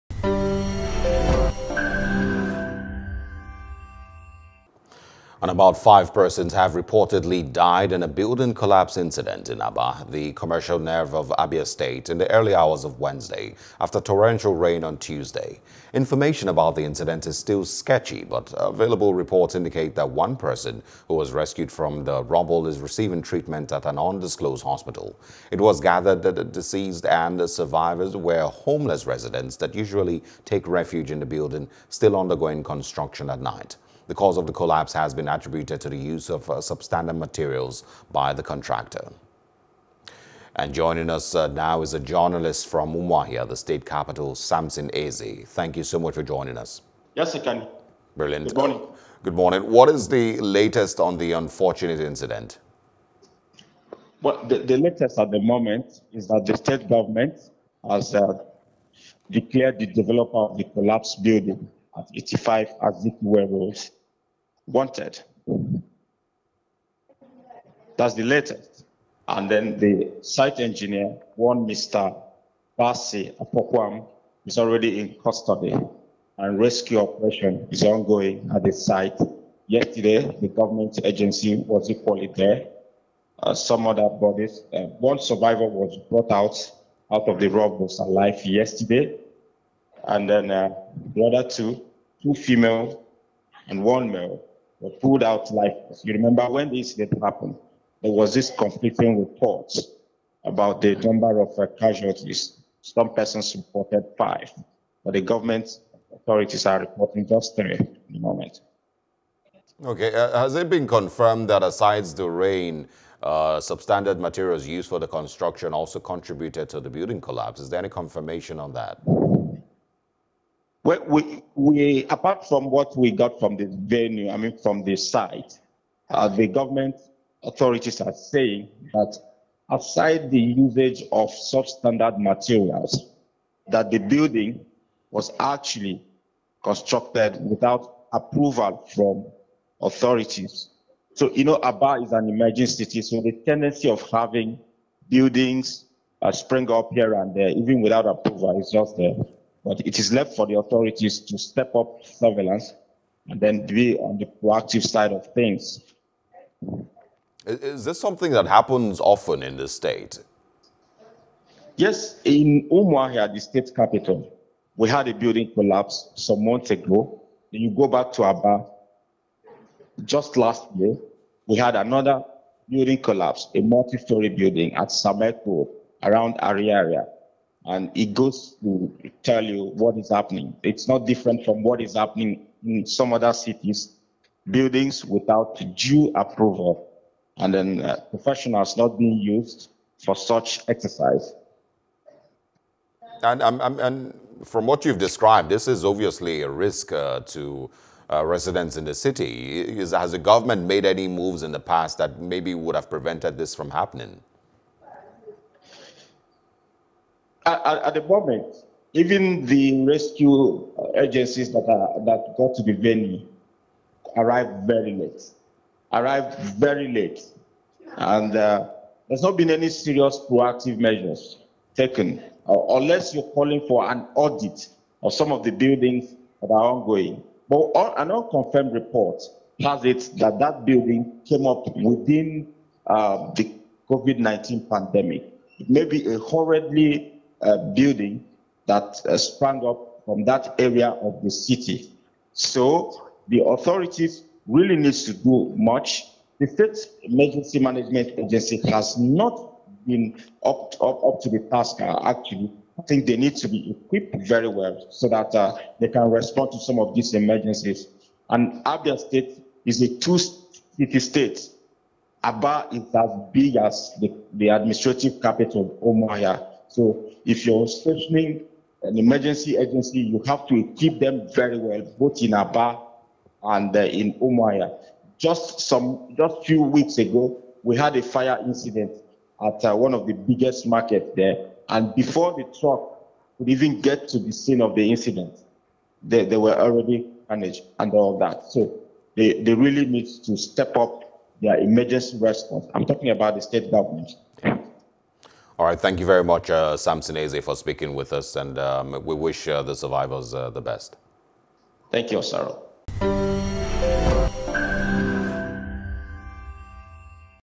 Abia Building Collapse: Five Feared Dead and One Rescued (NEWS | NIGERIA)
About five persons have reportedly died in a building collapse incident in Aba, the commercial nerve of Abia State in the early hours of Wednesday after torrential rain on Tuesday.